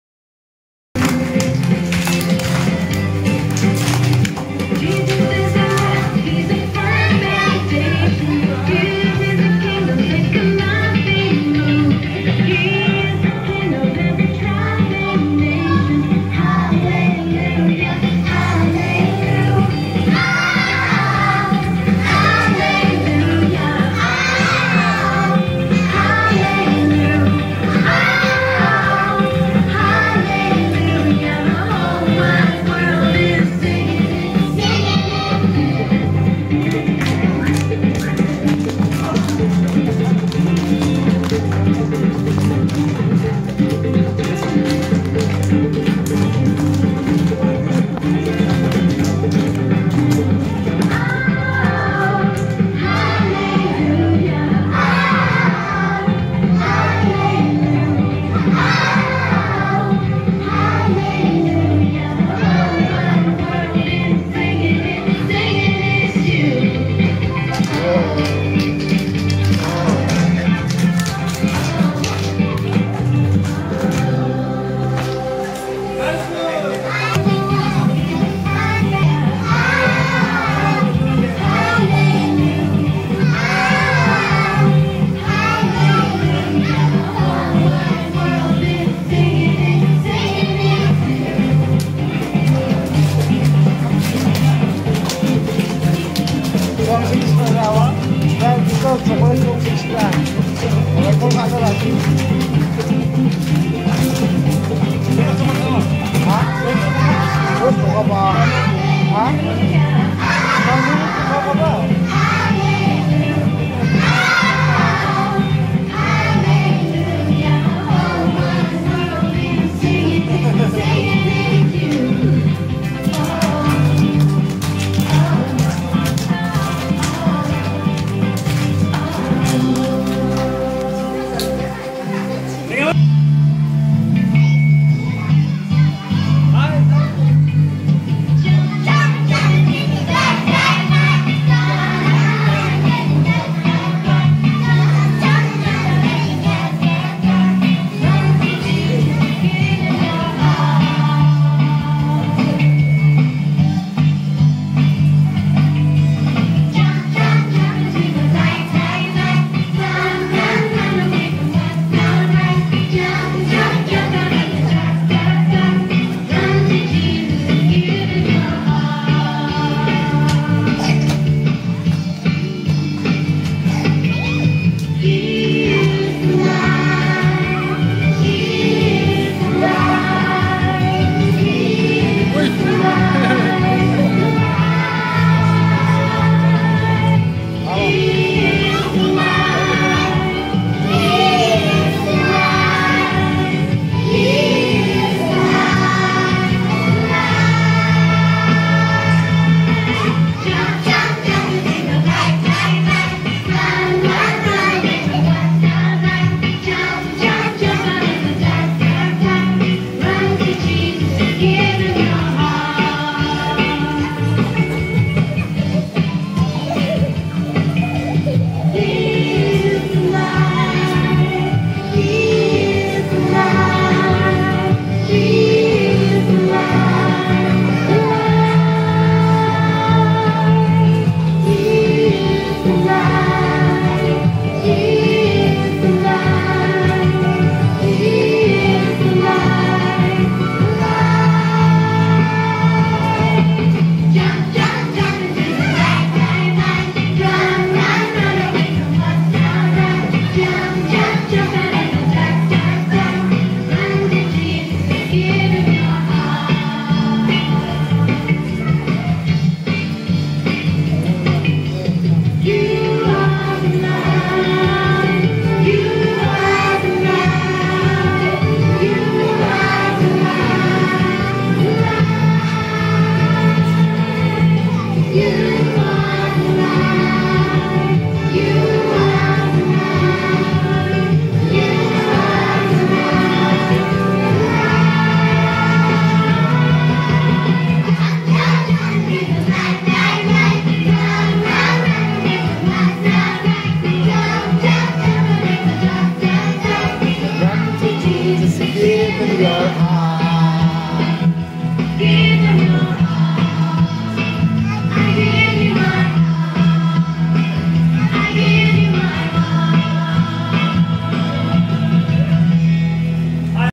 VBS Song | Action song | SONNU RTV
This video was taken during the vbs programme organised by Nongalbibra Baptist Church...Mag God bless this children💖💖💖💖💖
ia videoko nianichi sakantinan pattiani ongbachina.
Kasapae ia videoko share aro like kapachina molmolata. mitela